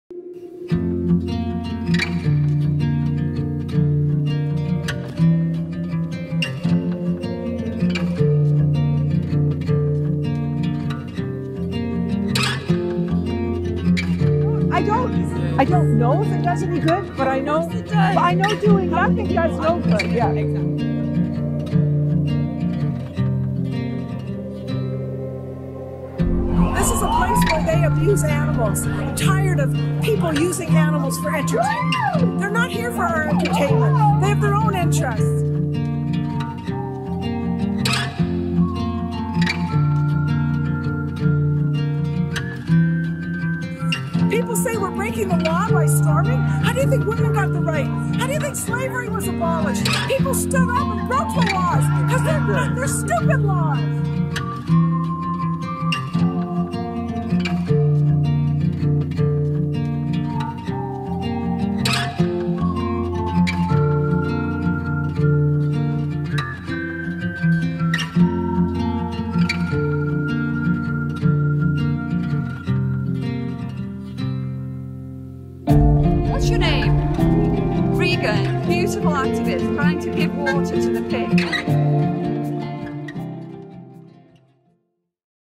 Live & Let Live | Mural in Honour of Regan Russell
During the Global Week of Action for Justice4Regan, Mar del Plata Animal Save's activists painted a mural to honour Regan and the pigs she was killed fighting for.

Stickering and postering were also done to raise awareness about who Regan was and what she believed in.

The whole world deserves to know the truth about what happened to her. We will not stop fighting until we get justice for Regan and empty all of the cages.